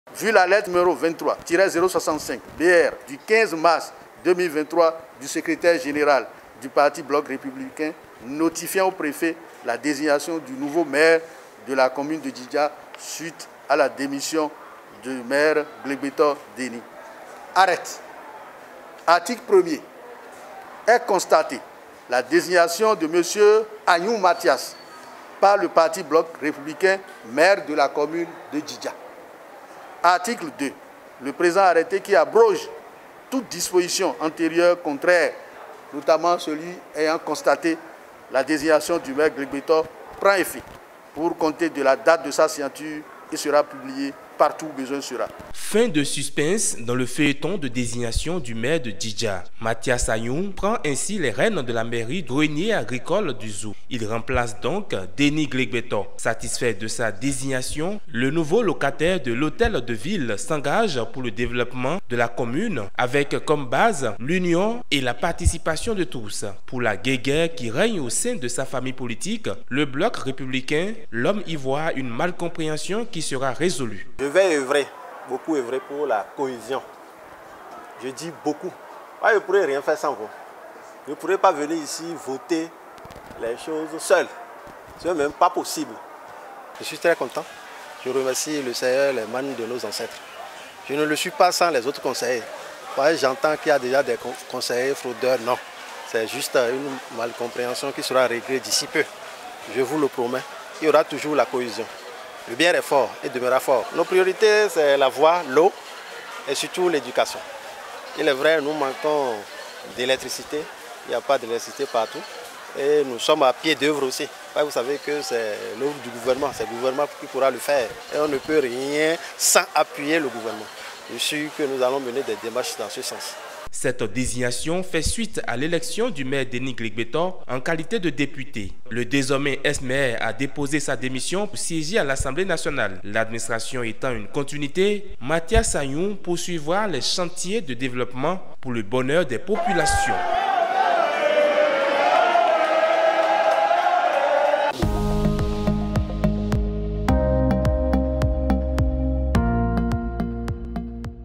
Mairie de Djidja : Matthias Agnoun remplace Denis Glégbèto à la tête de la commune
La commune de Djidja a un nouveau maire depuis ce lundi 03 Avril 2023. Il a pour nom Matthias Agnoun et a été dévoilé par le préfet du Zou Firmin Aimé Kouton aux élus communaux présents à la session extraordinaire